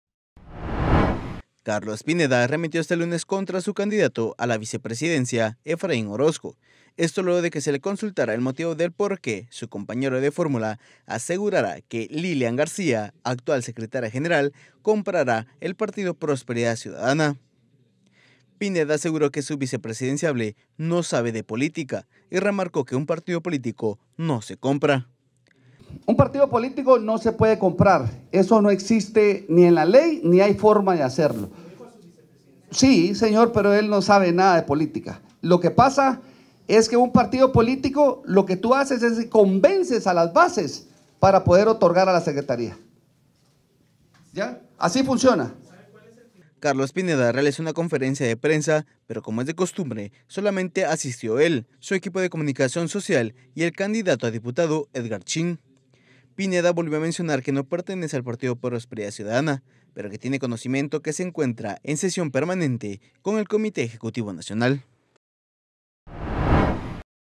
Carlos Pineda dice que su compañero de fórmula no sabe nada de política
Durante una conferencia de prensa, Carlos Pineda aseguró que su compañero de fórmula no sabe nada de política.